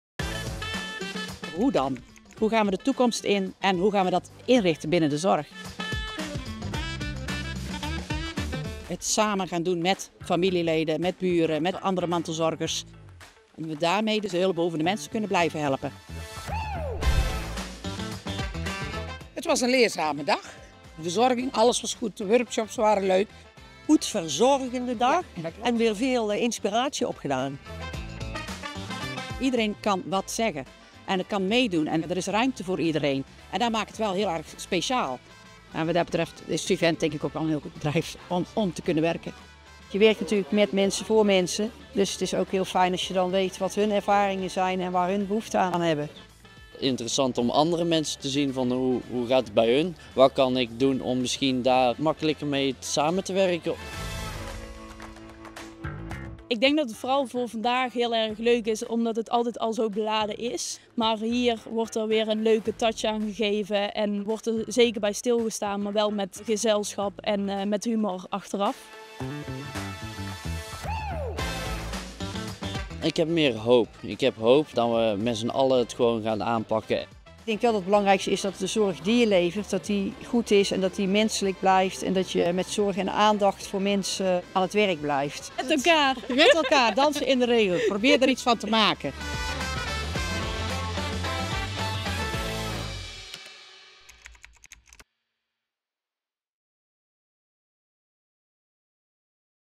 Viventdagen 2024 Aftermovie
Elk jaar vieren wij de Viventdagen. Een middag speciaal voor onze medewerkers, waarbij ontmoeten, kennis delen en inspiratie centraal staan. Een jaarlijkse mijlpaal in het aandacht geven aan elkaar en onze Vivent-trots, in Den Durpsherd in Berlicum. Het thema dit jaar was ‘Hoe dan?!'.

De wereld om ons heen verandert in een hoog tempo. Ook de wereld van de zorg voor ouderen en mensen die worden geconfronteerd met ziekte en chronische aandoeningen. Dus… de wereld van Vivent. Vivent gelooft dat we in de zorg grote stappen kunnen zetten door anders naar zorgvragen te kijken. Er écht zijn voor mensen die niet zonder zorg kunnen. Ons voor te bereiden op de toekomst.

De vraag naar zorg stijgt, maar het aantal collega’s niet. Er gaat dus iets veranderen. We zullen anders moeten kijken naar hoe we nu werken. Het anders doen. Welke keuzes maken we? En wat kun jij als medewerker hieraan bijdragen? Kortom, ‘Hoe dan?!’. Daarover gingen we met elkaar in gesprek en we volgden inspirerende workshops en presentaties.

De Viventdag eindigde met een lach, dankzij een leuk en erg energiek optreden van Mark van de Veerdonk, de Brabantse cabaretier. Zo sloten we de dag positief af en halen we altijd samen het beste uit de dag!